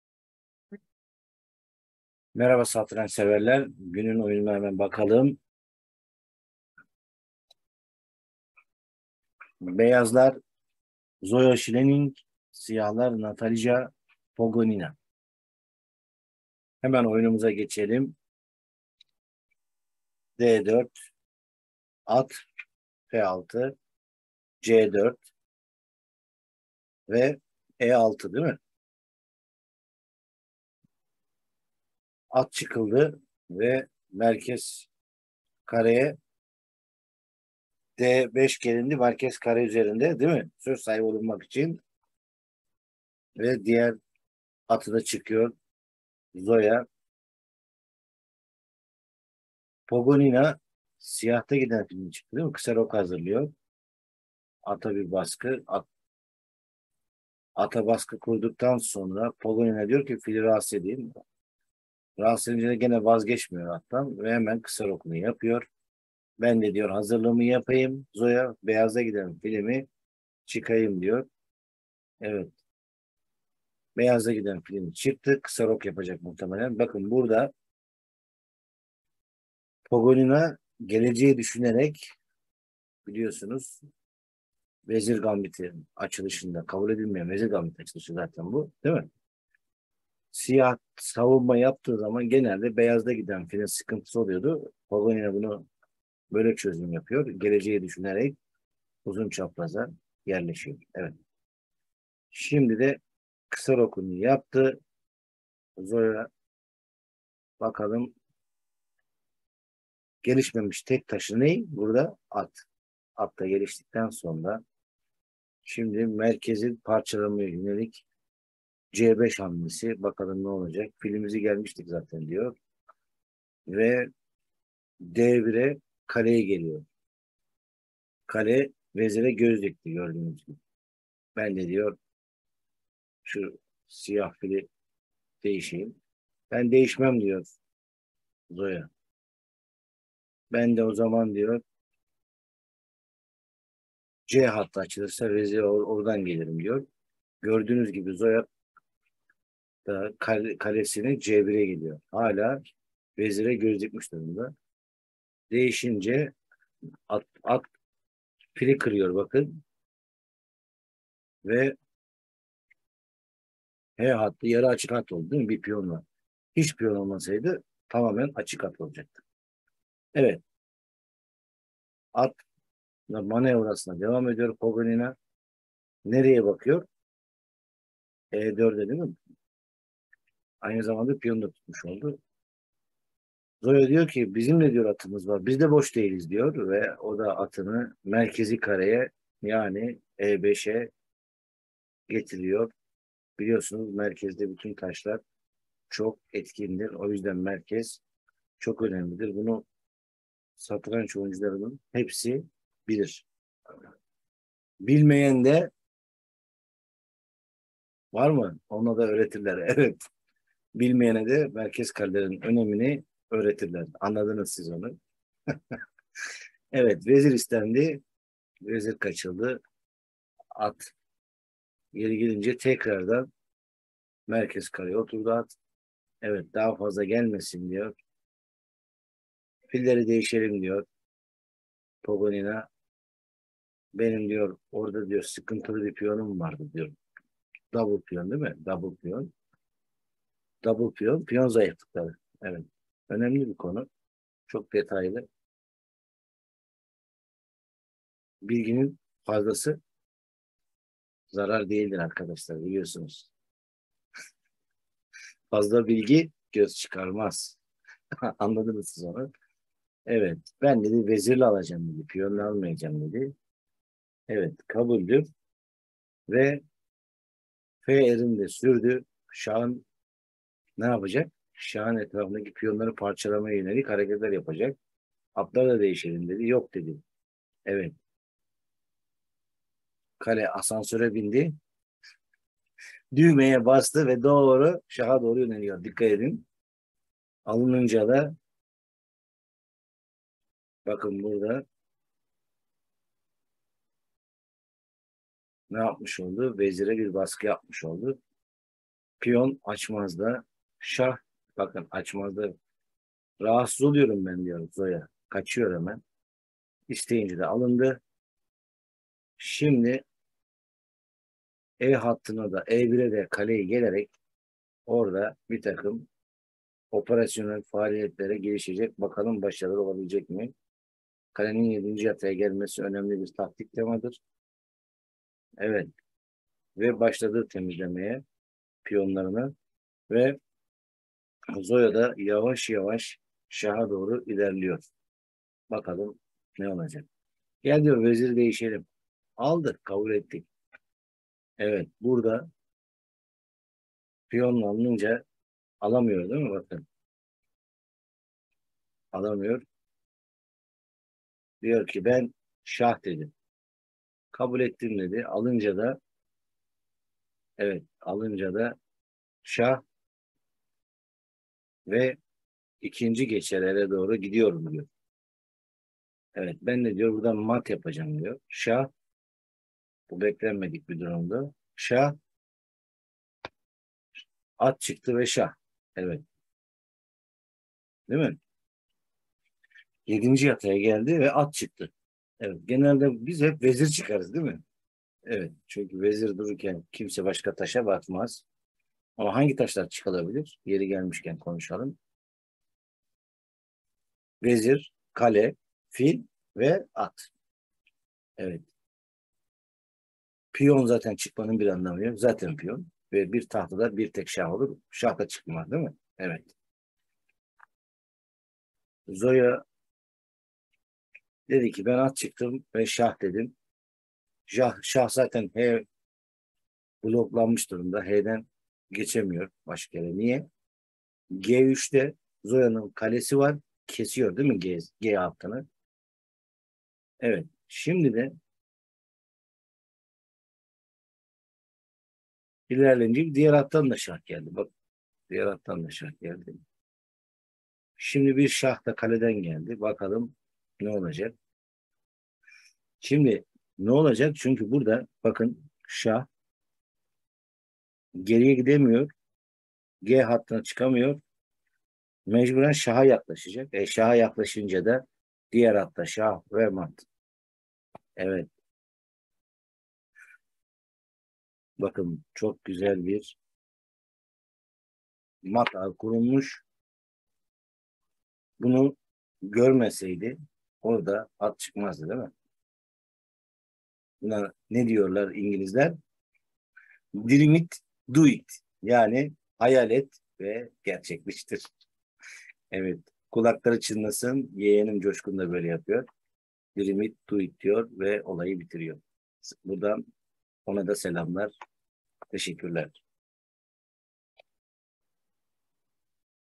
Zoya Schleining vs Natalija Pogonina"One Schleining Moment" (game of the day Jul-24-2022
Zoya Schleining vs Natalija Pogonina
"One Schleining Moment" (game of the day Jul-24-2022)
European Championship (Women) (2014), Plovdiv BUL, rd 8, Jul-14